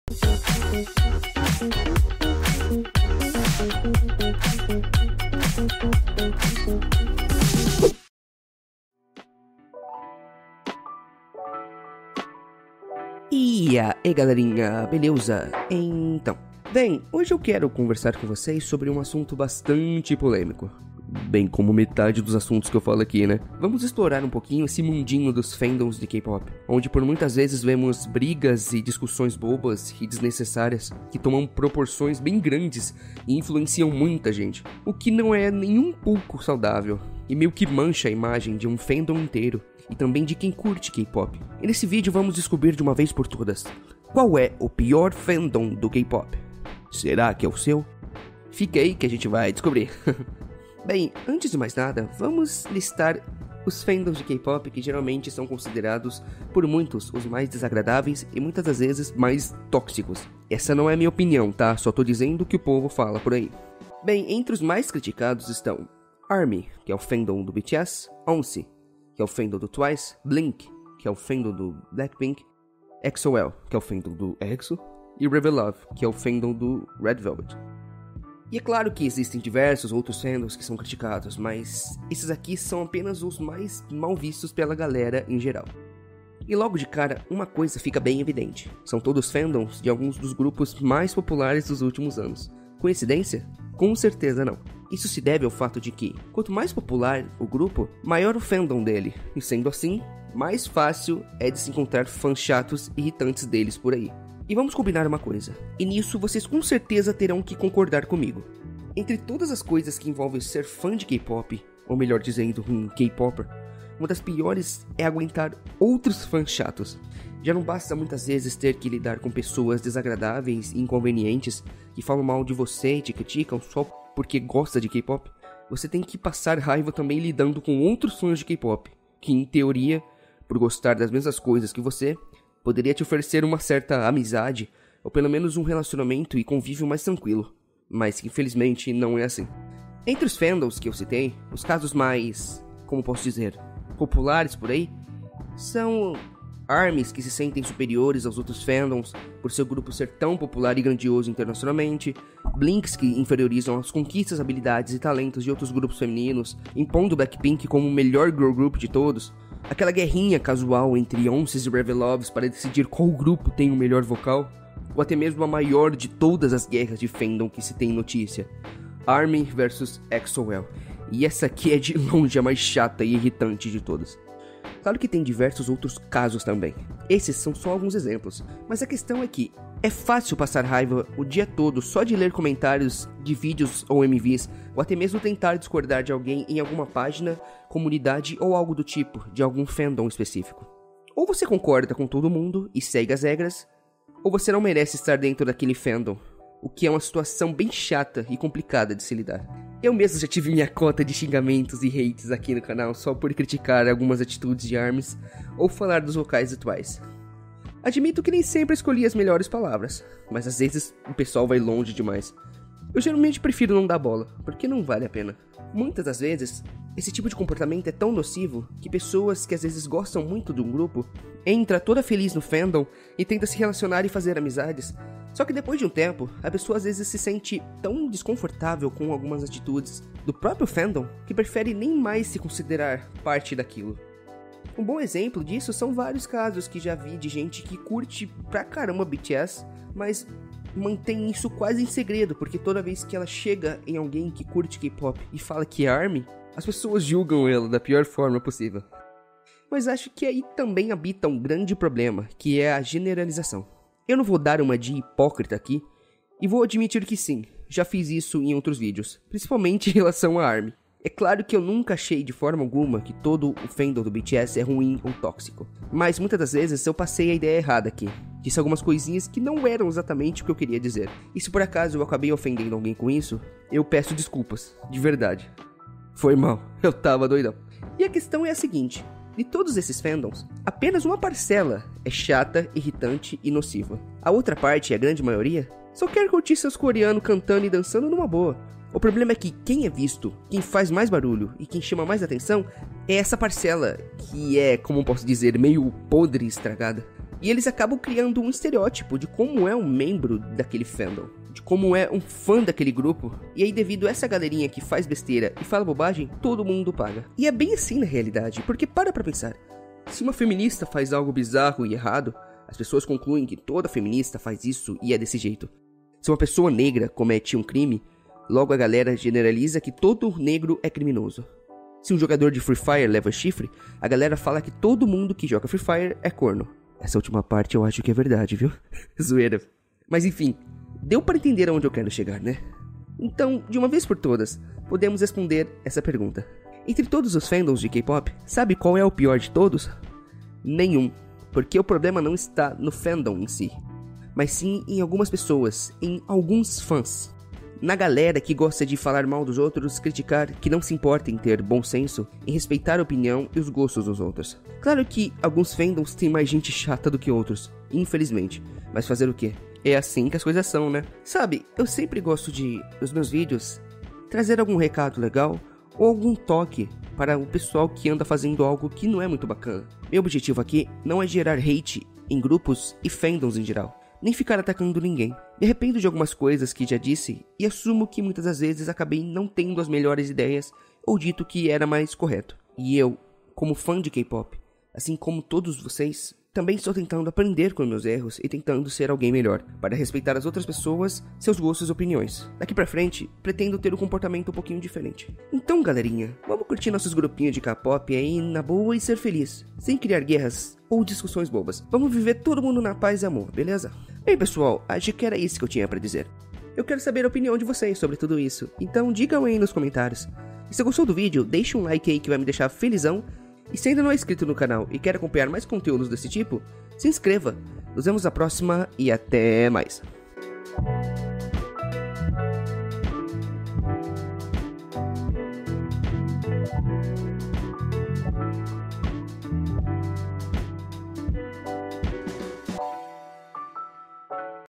Qual é o PIOR FANDOM do KPOP?
EAI GALERINHA!!

Nesse vídeo vamos discutir um pouco sobre a parte tóxica e ruim dos fandom de KPOP, e vamos descobrir de uma vez por todas: Qual é o pior fandom de todos?